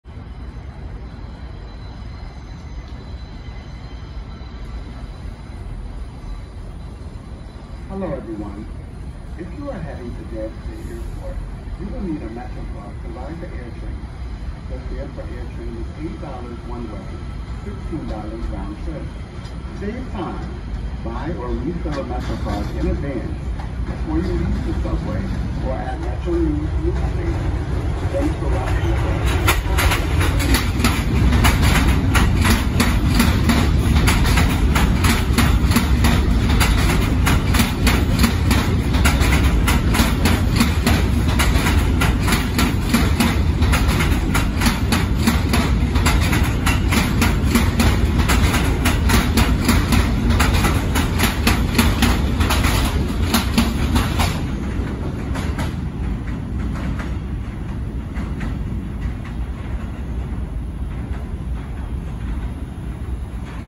NYC Subway R211A Train bypassing Rockaway Blvd (Mar. 16, 2023)
On the evening of March 16, 2023, I caught a R211A train that was not in service bypassing Rockaway Blvd station on the (A) line. Enjoy the nice catch!